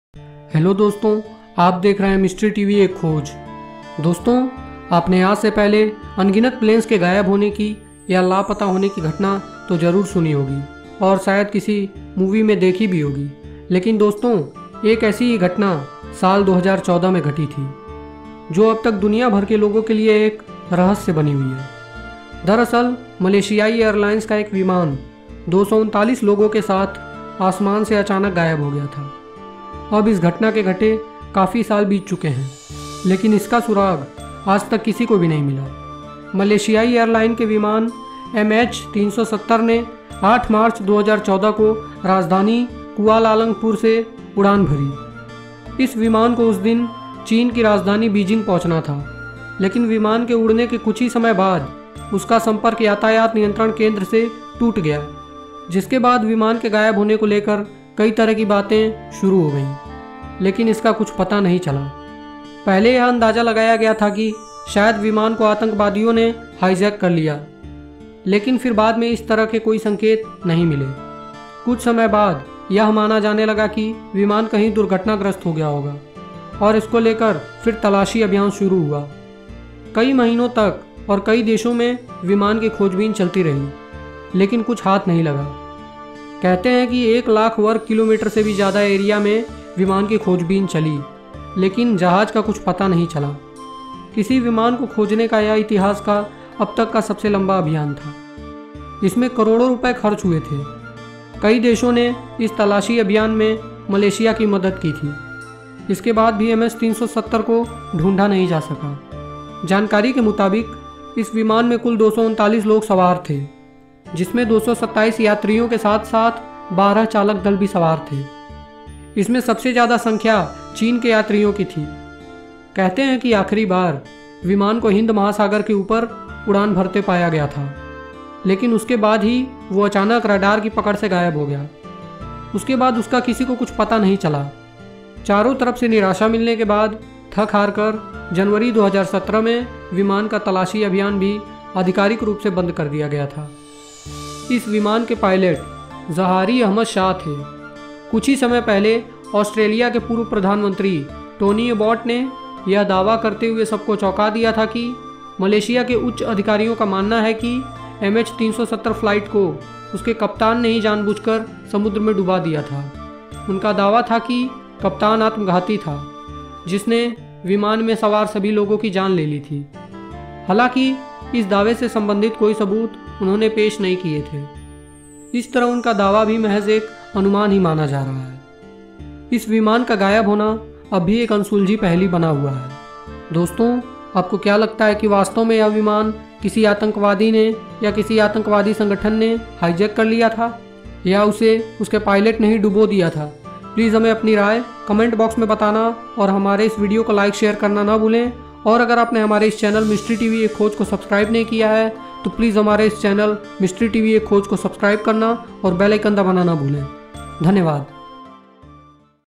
ये प्लेन 239 लोगों के साथ गायब हो गया||Flight MH 370 Ka Rahasya|Unsolved Mystery Of Flight MH 370
Malaysia Airlines Flight 370 (also known as MH370 or MAS370)was a scheduled international passenger flight operated by Malaysia Airlines that disappeared on 8 March 2014 while flying from Kuala Lumpur International Airport to its planned destination, Beijing Capital International Airport. The crew of the Boeing 777-200ER aircraft last communicated with air traffic control (ATC) around 38 minutes after takeoff when the flight was over the South China Sea. The aircraft was lost from ATC radar screens minutes later but was tracked by military radar for another hour, deviating westwards from its planned flight path, crossing the Malay Peninsula and Andaman Sea. It left radar range 200 nautical miles (370 km) northwest of Penang Island in northwestern Peninsular Malaysia. With all 227 passengers and 12 crew aboard presumed dead, the disappearance of Flight 370 was the deadliest incident involving a Boeing 777 and the deadliest in Malaysia Airlines' history until it was surpassed in both regards by Malaysia Airlines Flight 17, which was shot down while flying over conflict-stricken Eastern Ukraine four months later. The combined loss caused significant financial problems for Malaysia Airlines, which was renationalised by the Malaysian government in August 2014.
आज से 7 साल 8 महीने पहले साल 2014 में मलेशिया एयरलाइंस के एक विमान एमएच-370 के साथ जिसने 239 लोगों के साथ राजधानी कुआलालंपुर से चीन की राजधानी बीजिंग जाने के लिए उड़ान भरी लेकिन वह आसमान में ही कहीं गायब हो गया जिसका आजतक कुछ भी पता नहीं चला । विमान के उड़ने के कुछ ही समय बाद उसका संपर्क यातायात नियंत्रण केंद्र से टूट गया ।
इस विमान के बारे में, पहले यह अंदाजा लगाया गया कि शायद विमान को आतंकियों ने हाईजैक कर लिया होगा मगर फिर बाद में इस तरह के कोई संकेत नहीं मिले । इसके बाद फिर यह माना गया कि शायद विमान कहीं दुर्घटनाग्रस्त हो गया होगा लेकिन इसके भी कोई सबूत नहीं मिले । कहते हैं कि एक लाख वर्ग किलोमीटर से भी ज्यादा के दायरे में विमान की खोजबीन चली, लेकिन जहाज का कुछ पता नहीं चला ।किसी विमान को खोजने का यह इतिहास में अब तक का सबसे लंबा अभियान था। इसमें करोड़ों रुपये खर्च हुए थे । जानकारी के मुताबिक, इस विमान में कुल 239 लोग सवार थे, जिसमें 227 यात्रियों के साथ-साथ 12 चालक दल के सदस्य भी शामिल थे । इनमें सबसे ज्यादा संख्या चीन के यात्रियों की थी ।
Unsolved Mystery Of Flight MH 370
flight mh 370 ka rahasya
mh 370
fact story
unsolved mysteries